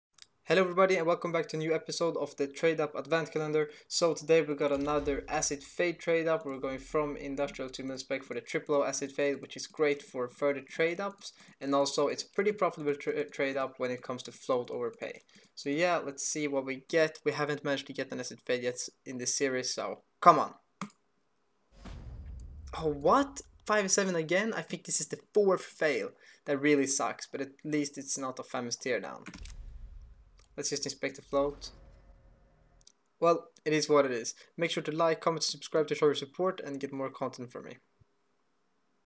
Tradeup Advent Calendar DAY 7
Hello everyone & welcome to the seventh episode of my christmas trade up contract advent calendar 2022 where I plan to do a tradeup everyday until the 25th of december!!!
I truly hope you enjoy the video and have nice day!

If you want to sign up for premium to see their premium tradeups, be able to track your tradeups and more.